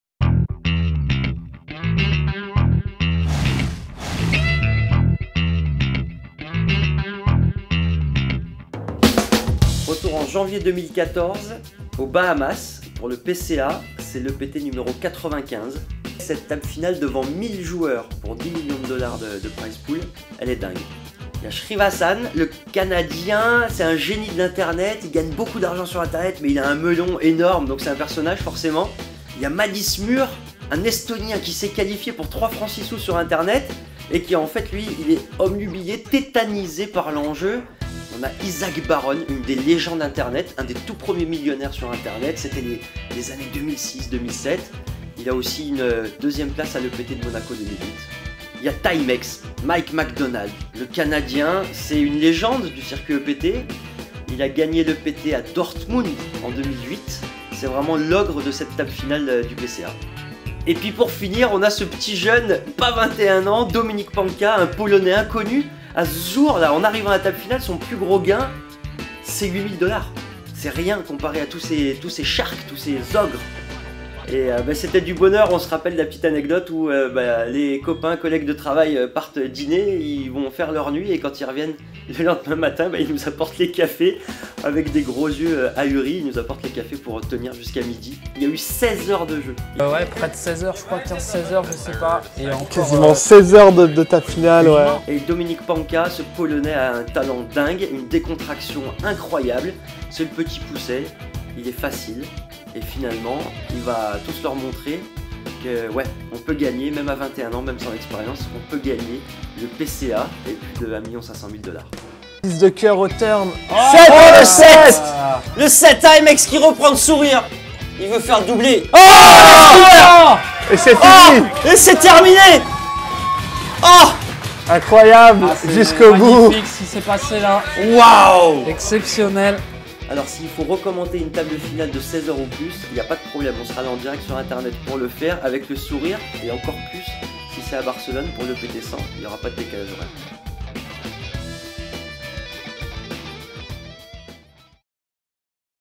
European Poker Tour : en route pour le 100ème ! Ep3
European Poker Tour : en route pour le 100ème !

Le 100ème European Poker Tour de l'histoire se jouera du 19 au 27 août prochain à Barcelone.

Retour sur quelques moments marquants de l'Histoire du plus grand cicruit de poker européen avec les équipes de commentateurs EPT de PokerStars.fr

EPT 95 au Bahamas - 2014
Benjamin Domenget revient sur une table finale de rêve.
Entre quête du doublé EPT (Mike MacDonald), d'une belle transition Online vers le Live (Shyam Srinivasan), du rêve d'une vie (Madis Müür), d'une confirmation (Isaac Baron) ou d'une révélation (Dominik Panka), il leur aura fallu plus de 16h00 de jeu pour se départager !

Rendez vous dès le 19 août sur PokerStarsLive pour suivre l'EPT 100 Barcelone en direct !